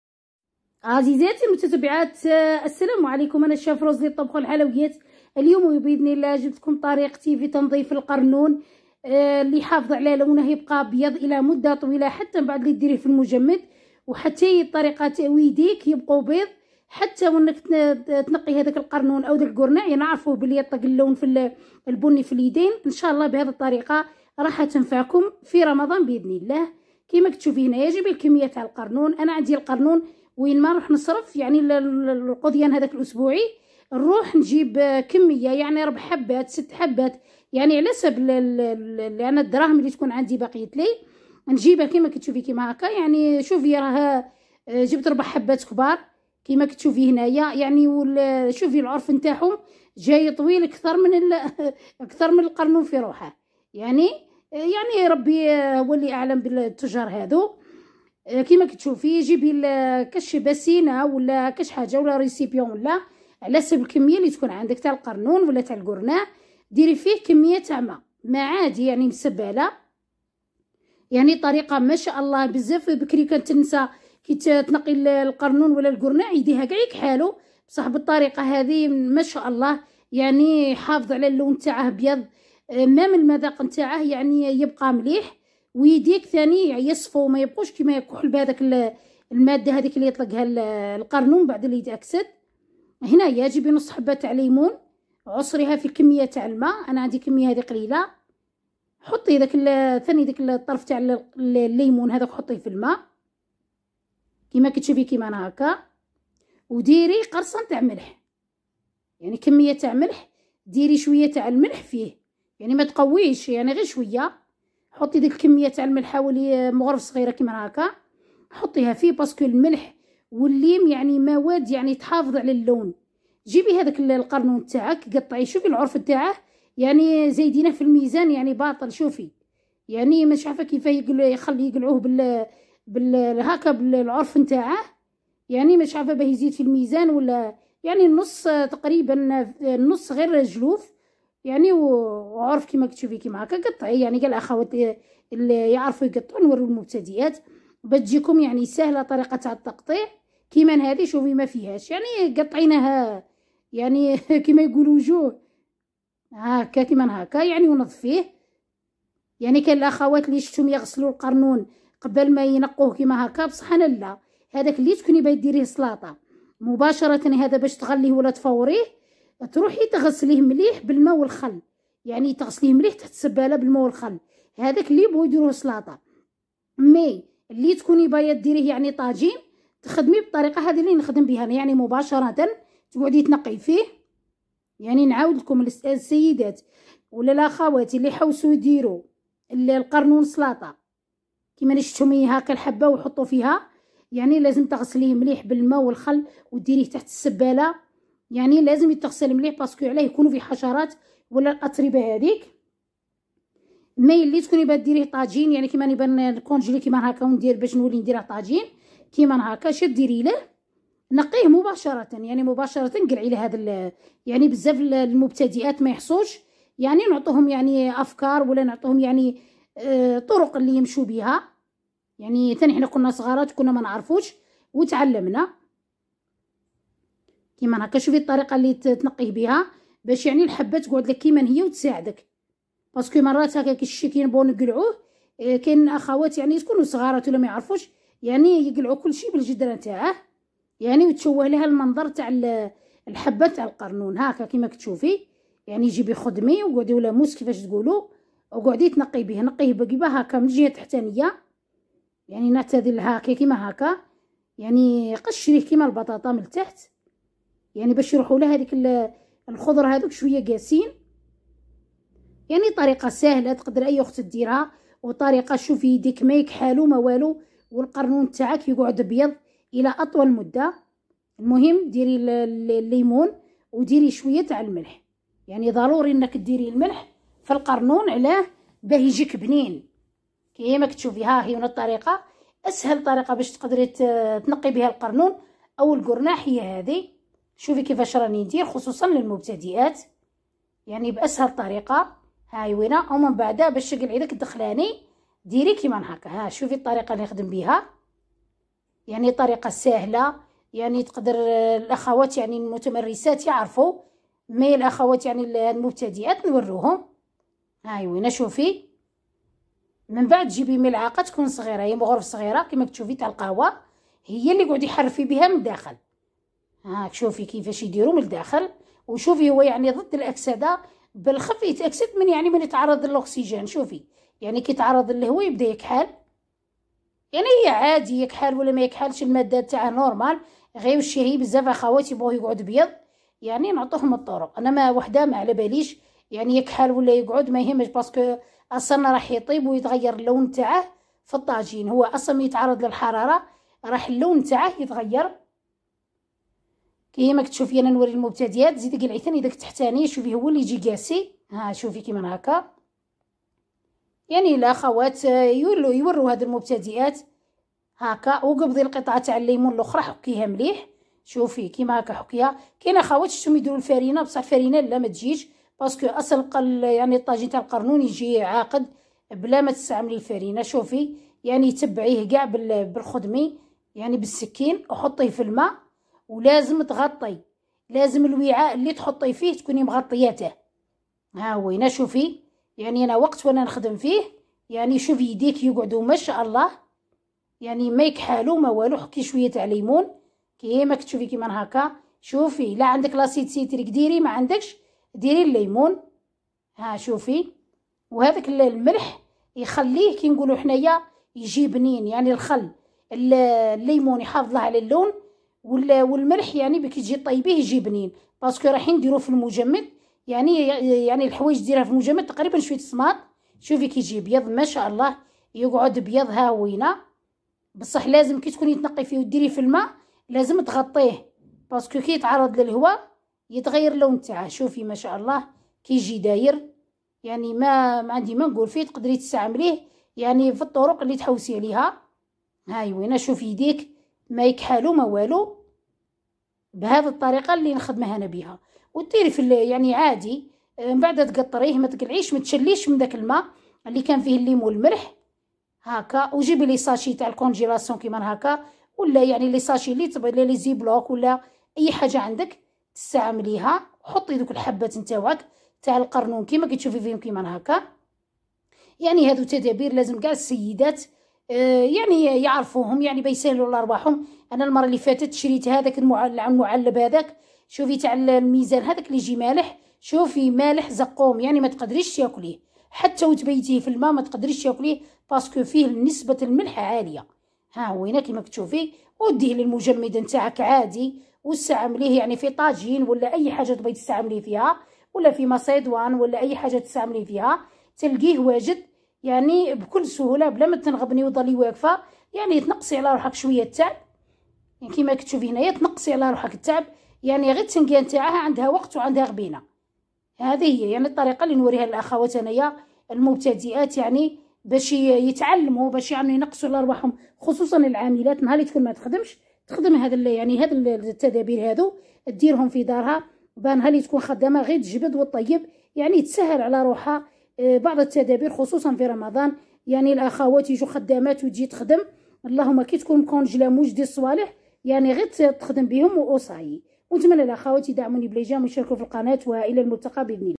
رمضان 2024.اسهل طريقة تنظيف القرنون وسر بقائه ابيض والاحتفاظ به لاطول مدة للمبتدئات/تحضيرات_رمضان
رمضان 2024.طريقة تنظيف القرنون وسر بقائه ابيض والاحتفاظ به لاطول مدة للمبتدئات
السلام عليكم ورحمة الله وبركاتهفي هذا الفيديو نقدم لكم طريقة تنظيف القرنون أو الأرضي شوكي مع سر الحفاظ على لونه وبقائه أبيض والاحتفاظ به في المجمد .. نتمنى أنو الطريقة تفيدكم.طريقة تنظيف القرنون/الأرضي شوكي/الخرشوف شرح مفصل للمبتدئات و سر بقائه أبيض و ناصع كالثلج.وكيفية  تنظيف القرنون و سر بقائه أبيض ناصع| طريقة الإحتفاظ به لأكثر من عام
ممكن تصبروه لأنو كاين لي ما يحبوش المجمد .. تقدرو تحتافظو بيه في قنينات زجاجية معقمة وبعد ما تعمروهم بالقرنون تضيفو الماء مع القليل من الخل وتحطوهم في ماء (حتى يتغطاو) يغليو 10د وتخليوهم يبردو مليح وتحتافظو بيهم في مكان جاف وبعيدا عن الضوء لمدة سنة كاملة ما يصرالهم والو (من الاحسن استهلاك الكمية لي فتحتوها في أقرب وقت) .. أنا شخصيا نفضل نكونجليهم

شاف-روز-لطبخ-و-الحلويات